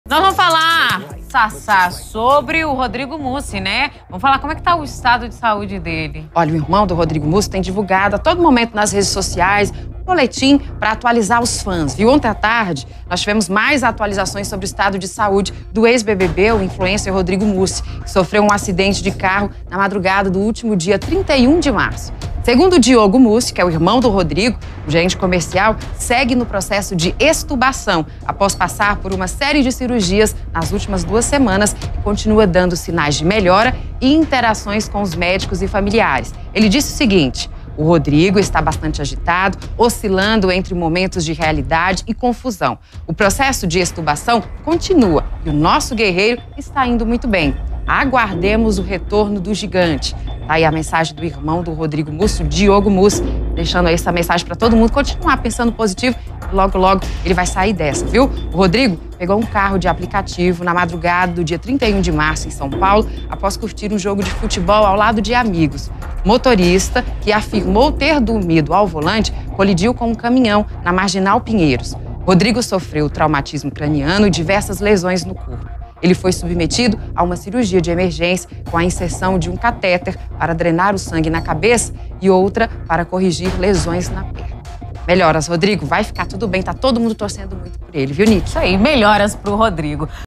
Rodrigo Mussi segue reagindo bem ao processo de extubação, diz irmão
O ex-BBB Rodrigo Mussi está avançando no processo de extubação. As informações foram divulgadas pelo irmão do ex-BBB e gerente comercial, Diogo Mussi. Segundo ele, Mussi está agitado e oscilando entre momentos de realidade e confusão, mas segue dando sinais de melhora e interações com os médicos e familiares.